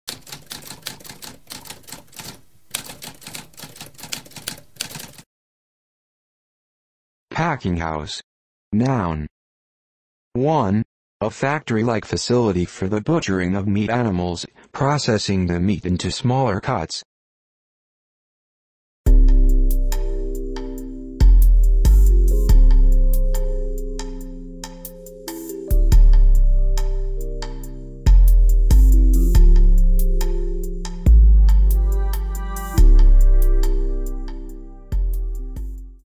What does packinghouse mean?
A spoken definition of packinghouse.

Intro Sound:
Typewriter - Tamskp